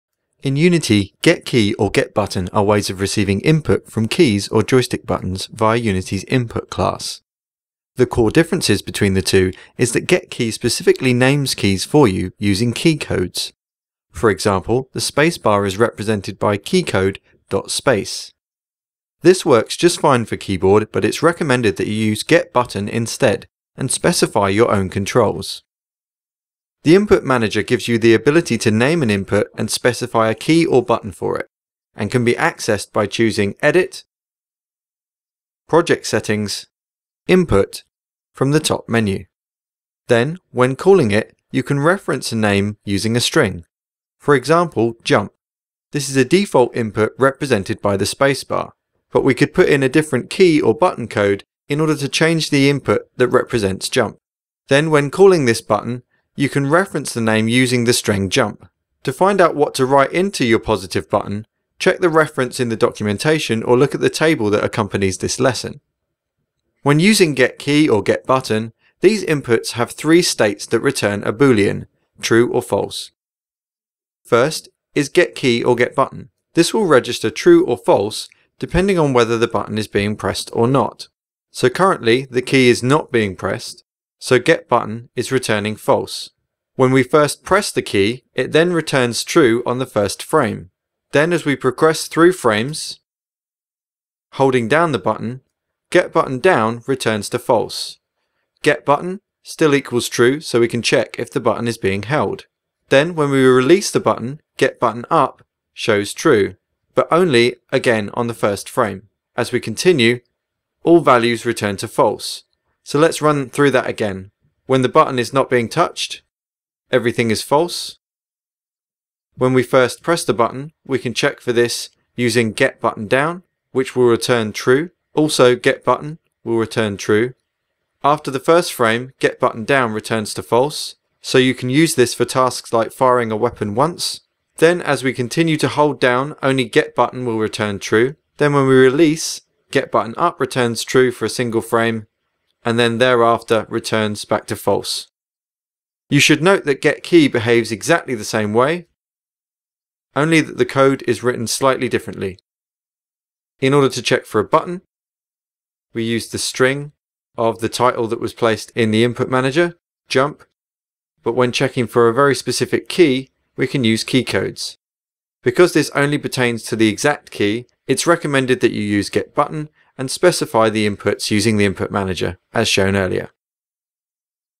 Input - GetButton and GetKey - Unity Official Tutorials
How to get button or key for input and how these axes behave / can be modified with the Input manager